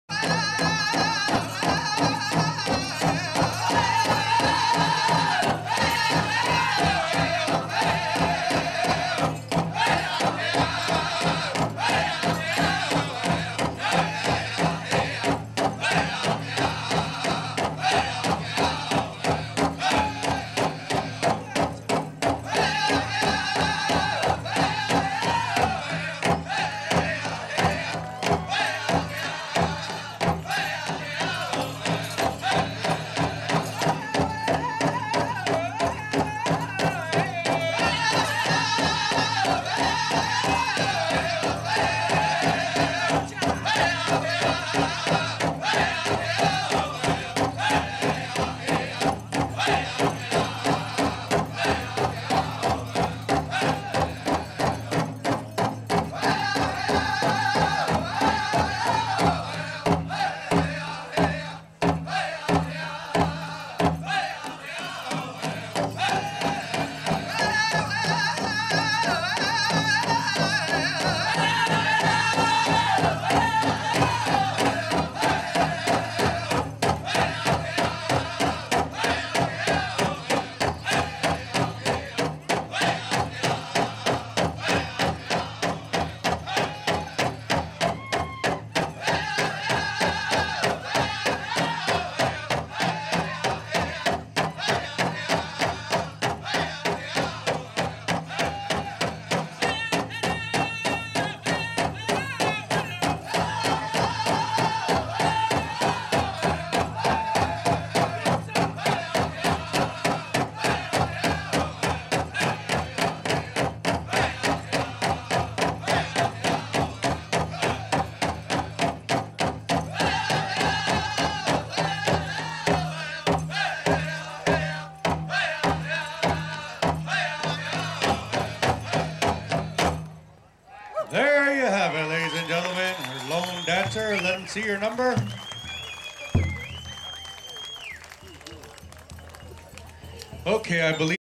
Teen Boys Fancy BEST C.R.I.T. Powwow 2011 Parker AZ
2453.  By YouTube simian1842.  Teen Boys Fancy.  Colorado River Indian Tribes Powwow 2011.